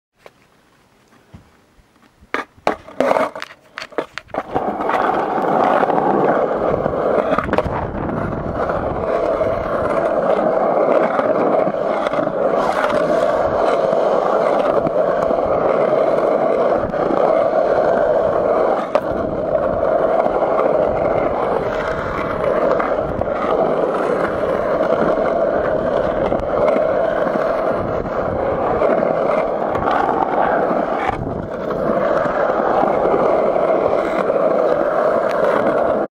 Orcas Island Skatepark Tour
Quick skate around the perimeter.